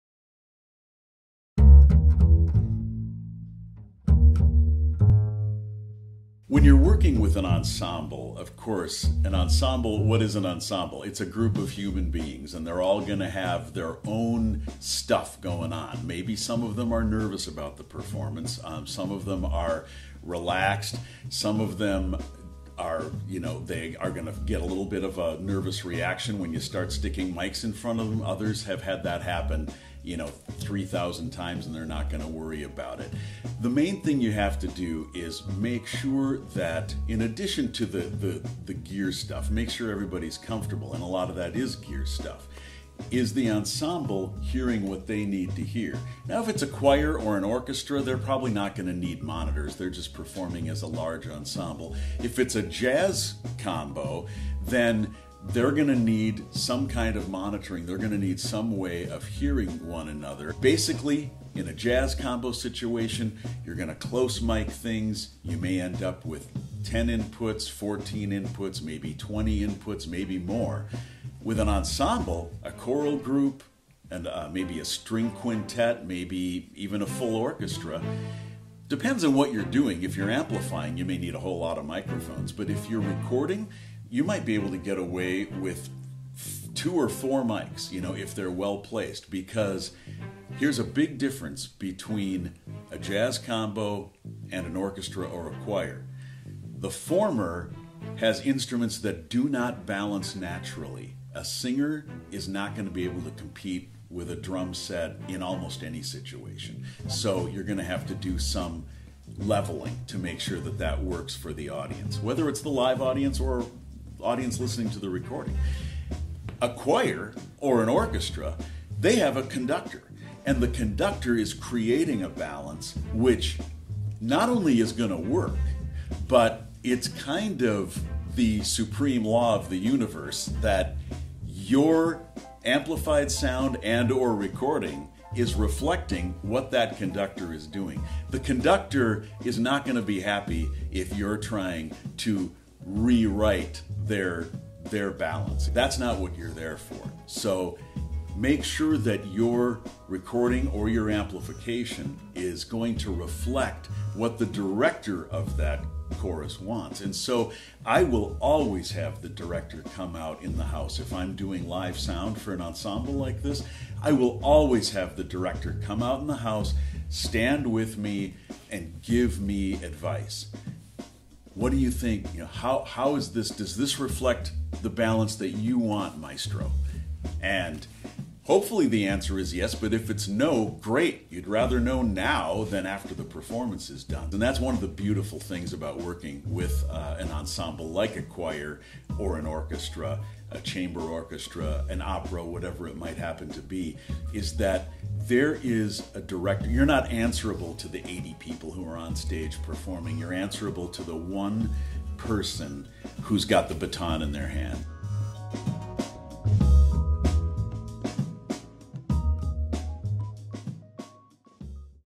How To Record Ensembles
From microphone placement to balancing levels to navigating personalities, recording ensembles can be a tricky endeavor.  Producer and studio co-owner Buzz Kemper has garnered a reputation for getting the best live and studio sound out of small and large ensembles, including orchestras, choirs and jazz combos.

and see why we are the premier provider of professional audio, lighting and video gear. Get Equipped!